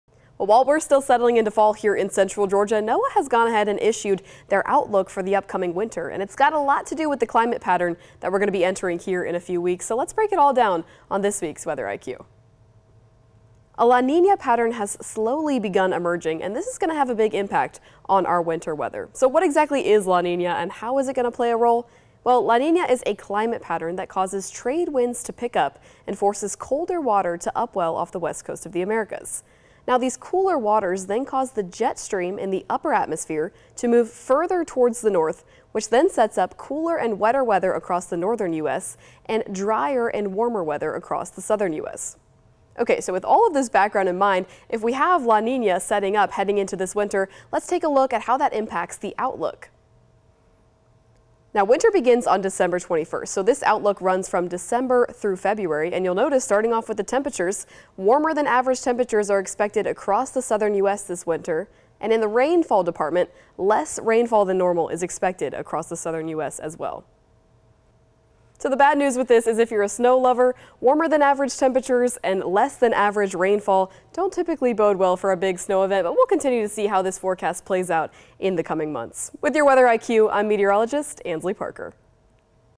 Weather IQ: What's the expected winter forecast?
NOAA has issued their official outlook for precipitation and rainfall during the winter season. Meteorologist Ansley Parker breaks it down.